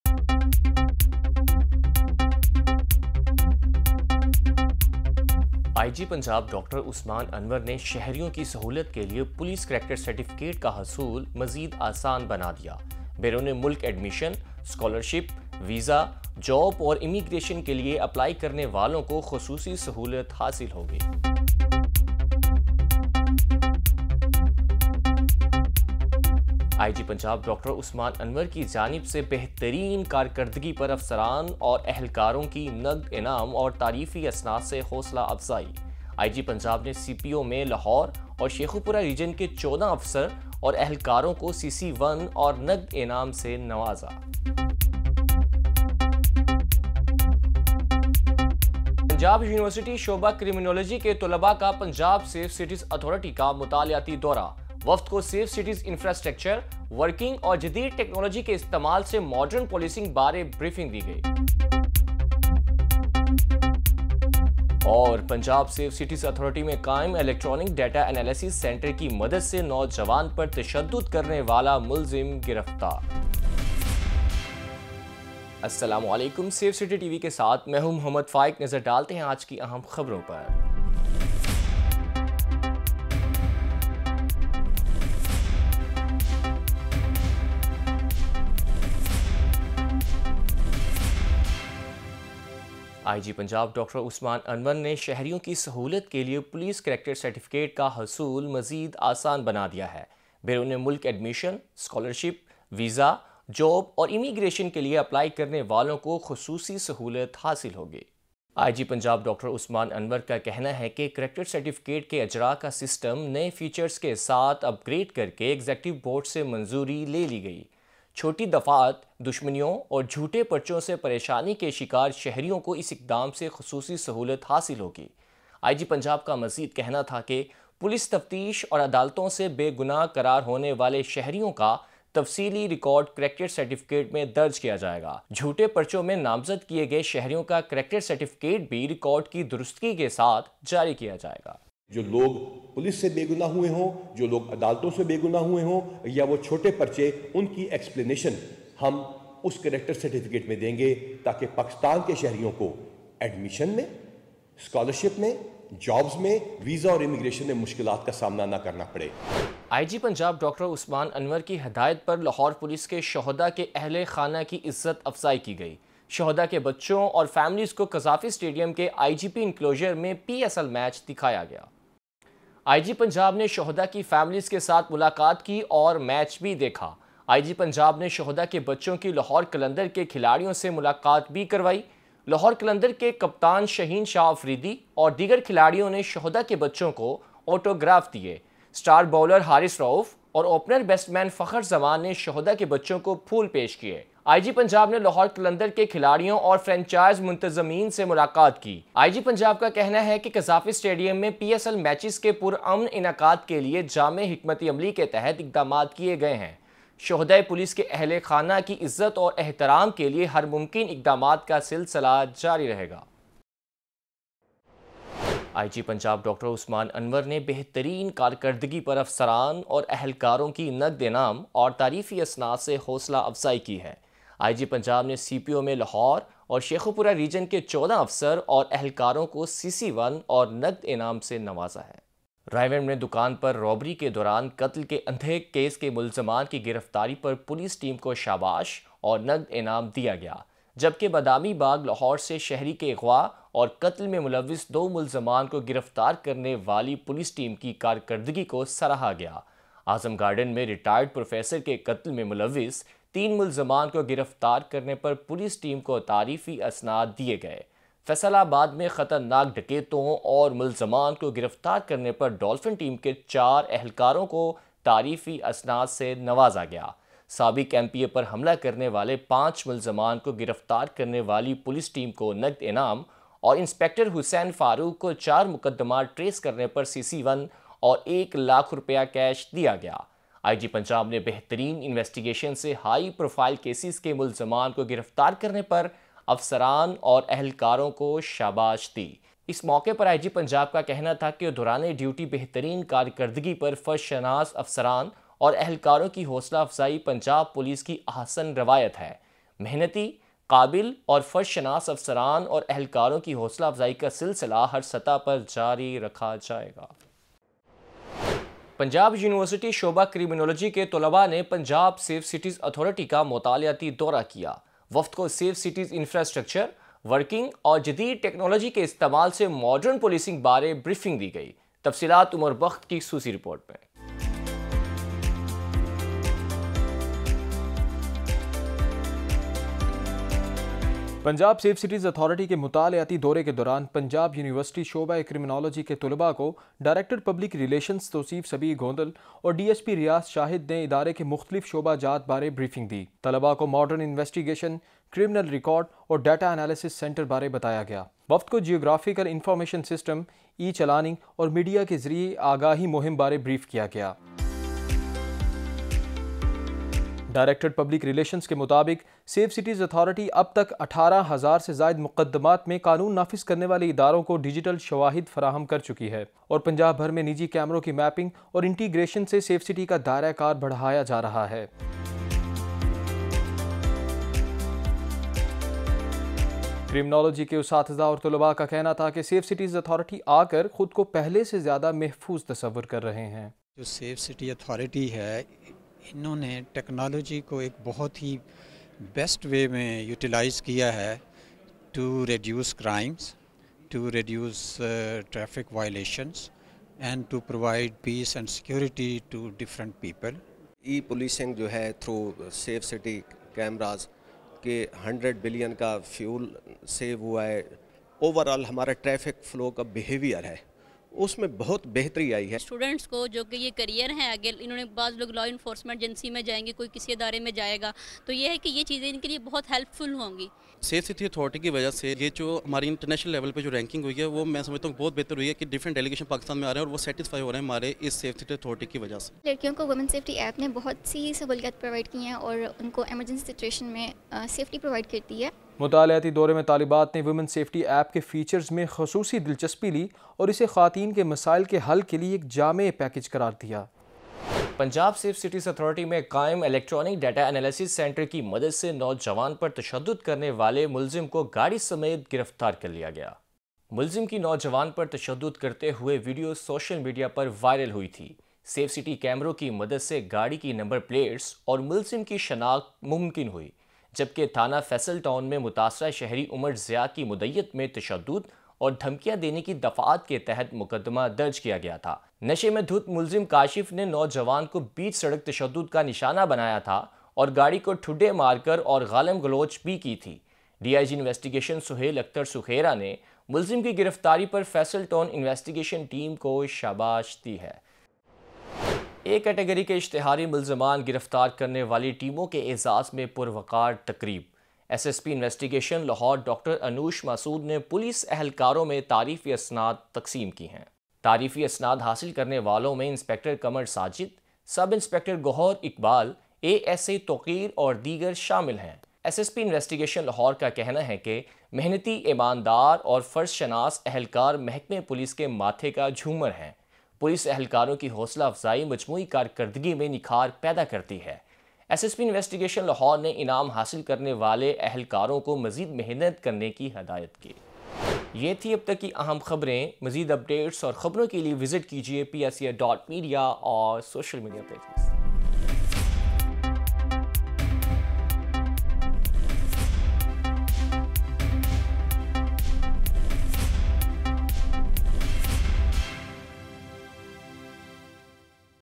IG Punjab has made more easier the issuance of police character certificates | PSCA Tv | 28 Feb 23
IG Punjab has made more easier the issuance of police character certificates. Honoring the families of the martyrs of Lahore Police on the instructions of IG Punjab. IG Punjab Dr. Usman Anwar encouraged officers and workers for their excellent performance with cash prizes and certificates of appreciation. Punjab University Department of Criminology students study visit to Punjab Safe Cities Authority. The accused of torturing the youth was arrested with the help of the electronic data analysis center established in the Punjab Safe State Authority. Details in the special bulletin of Safe City TV | 28 FEB 23